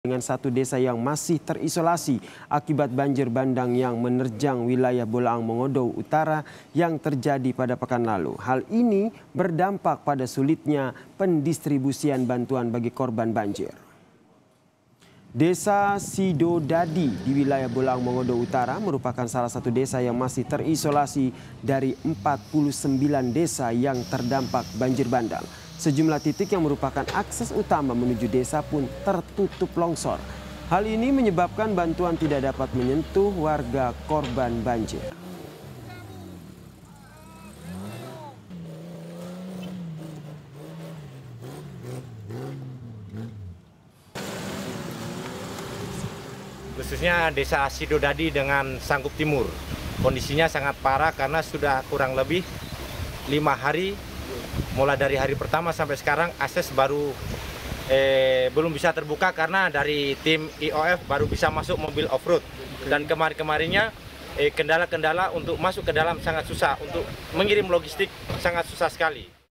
Akibat Banjir Bandang Pekan Lalu, 1 Desa Masih Terisolasi
SULAWESI UTURA, KOMPAS.TV - Satu desa masih terisolasi akibat banjir bandang yang menerjang wilayah Bolaang Mangondow Utara yang terjadi pada pekan lalu.

Hal ini berdampak pada sulitnya pendistribusian bantuan bagi korban banjir.

Desa Sidodadi di wilayah Bolaang Mangondouw Utara merupakan satu desa yang masih terisolasi dari 49 desa yang terdampak banjir bandang.

Sejumlah titik yang merupakan akses utama menuju desa tertutup longsor.

Hal ini menyebabkan bantuan tidak dapat menyentuh warga korban banjir.

Hingga Minggu (08/03/2020) sore, alat berat yang diturunkan masih berupaya untuk membuka akses jalan.

Upaya untuk masuk ke wilayah Desa Sidodai yang terisolasipun di tempuh dengan menggunakan kendaraan offrad milik iof, Sulawesi Utara.

Hingga kemarin, 4 kendaraan offroad, telah berhasil menembus desa sidodadi yang terisolasi, walau harus menembus medan yang berbahaya.

Banjir bandang menerjang wilayah Bolaang Mangondow, Sulawesi Utara pada Rabu, 4 Maret 2020 lalu.

Ratusan rumah dari puluhan desa, rusak parah diterjang banjir bandang yang membawa material tanah, batu, dan kayu.

Data dari pihak pemerintah daerah kabupaten, dua desa terparah dari banjir bandang yaitu Desa Domisil dan Desa Pangi Timur.

Satu korban tewas dalam peristiwa ini, merupakan seorang anak berumur 5 tahun.